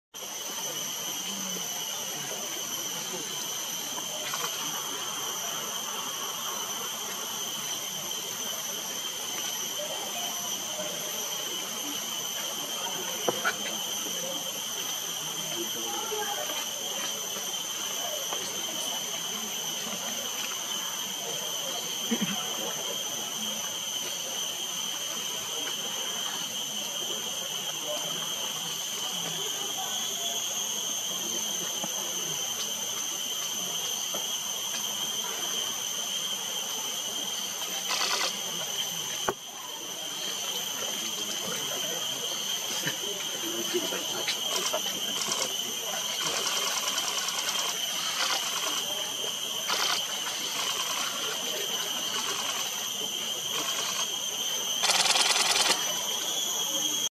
Black Bittern, SBWR
Black Bittern fishing in SBWR. 20 Nov 2018